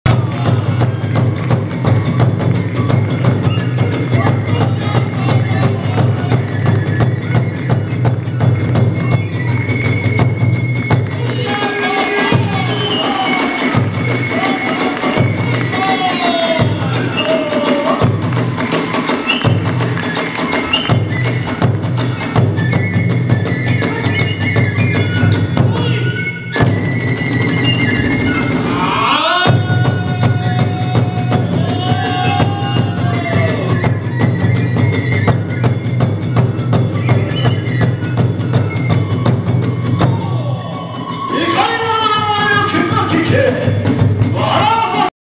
あさきた神楽公演第3回 綾西神楽団「葛城山Ⅳ」
平成２９年７月１７日（月）
広島市安佐北区民文化センター大ホール
あさきた神楽公演第３回
綾西神楽団「葛城山Ⅳ」

綾西神楽団様に撮影許可と公開の許諾いただいています。